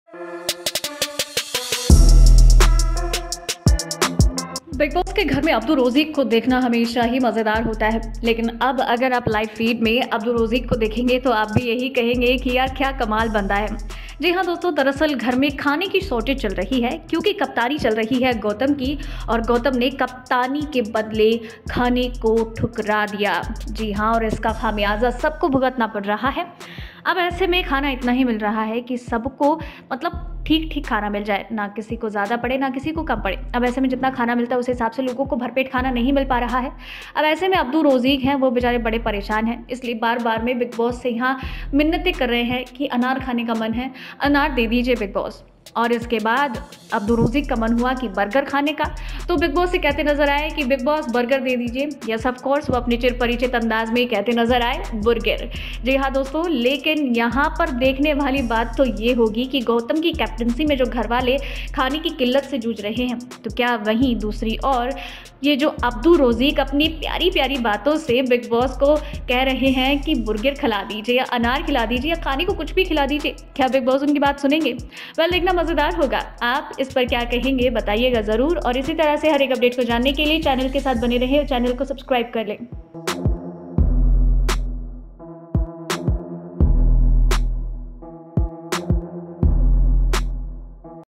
Abdu Rozik ने BiggBoss से की ये खास डिमांड !
Bollywood Planet ( BP ) : Bigg Boss16 : Abdu Rozik Demands For Food From Bigg Boss | Abdu Rozik Latest News & Updates

Edited By -